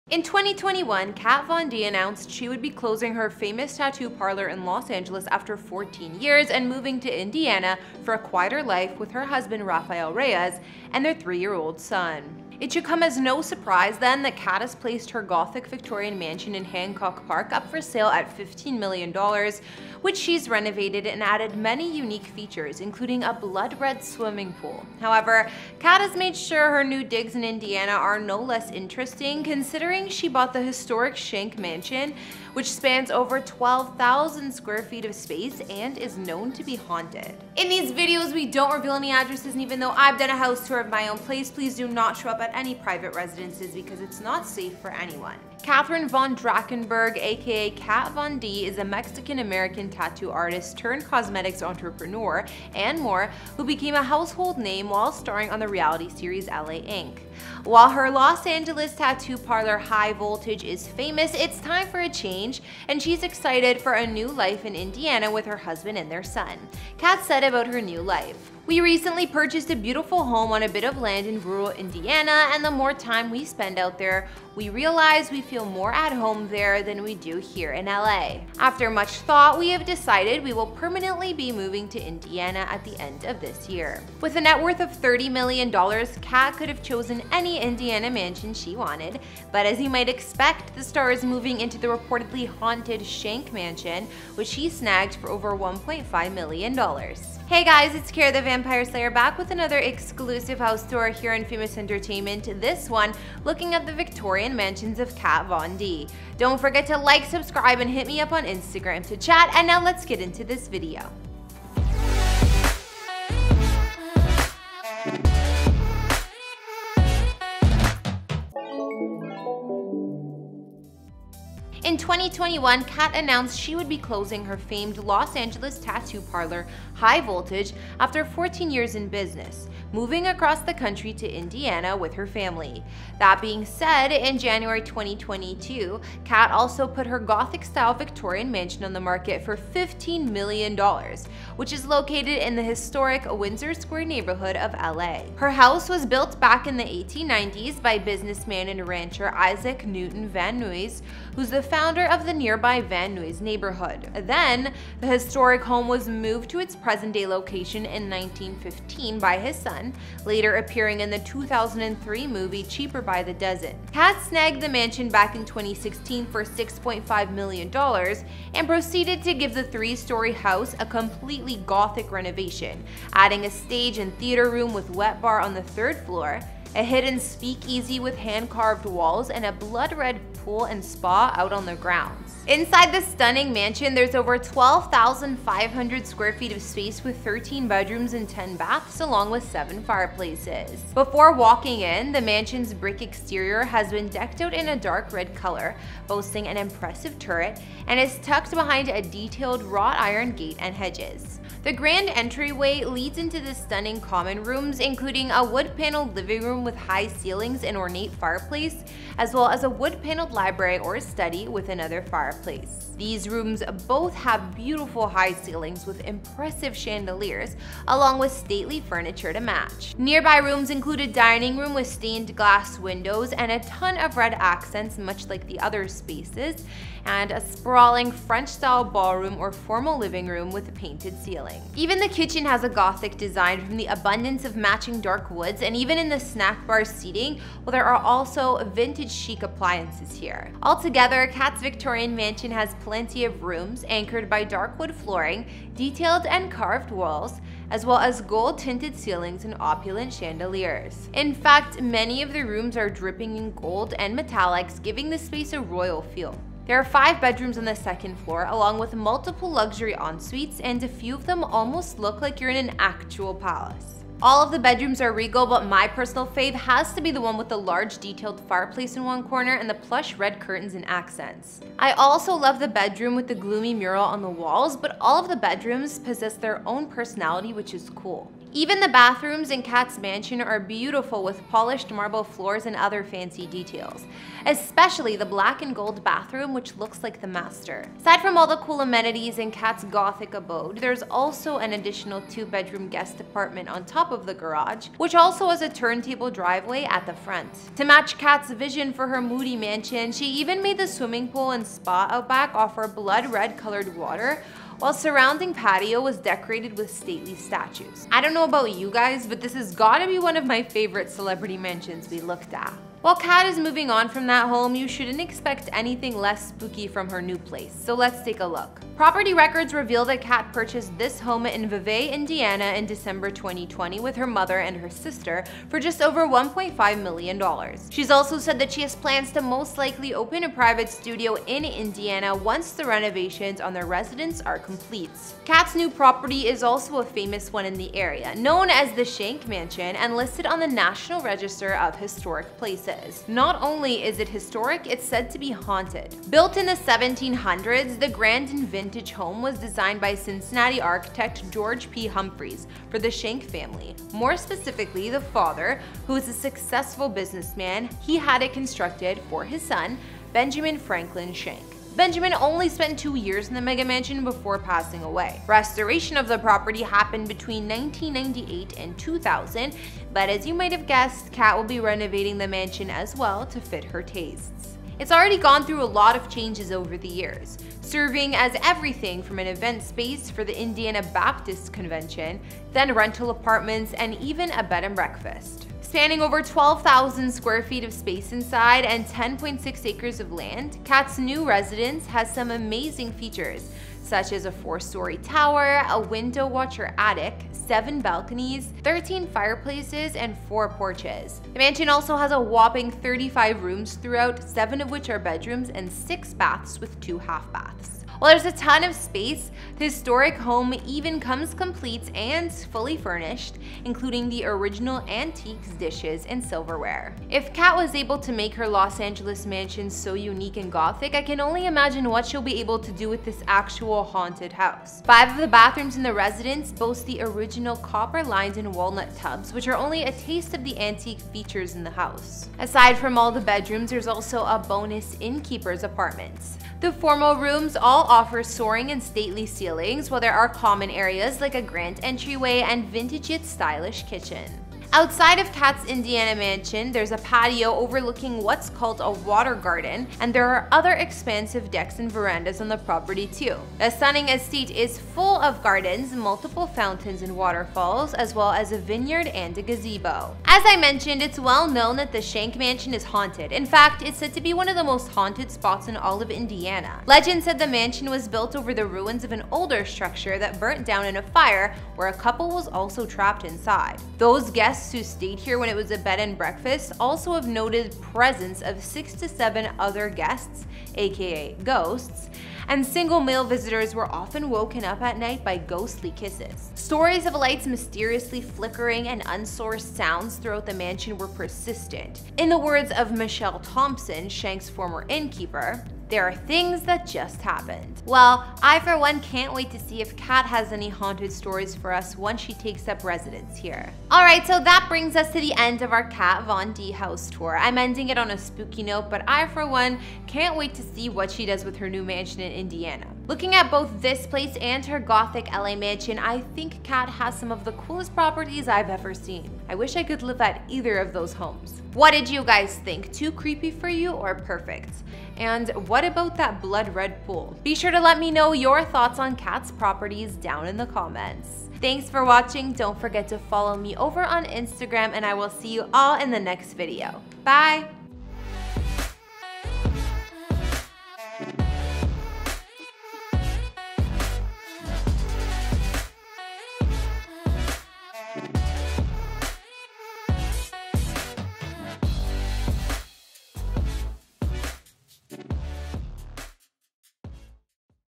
Kat Von D | House Tour | $15 Million Indiana Mansion & More
In 2021 Kat Von D announced she would be closing her famous tattoo parlor in Los Angeles after 14 years, and moving to Indiana for a quieter life with her husband Rafael Reyes and their 3-year-old son. It should come as no surprise then that Kat has placed her gothic-Victorian mansion in Hancock Park up for sale at $15 million which she’s renovated and added many unique features – including a blood-red swimming pool. However, Kat has made sure her new digs in Indiana are no less interesting considering she bought the historic Schenck mansion there which spans over 12,000 square feet of space and is known to be haunted.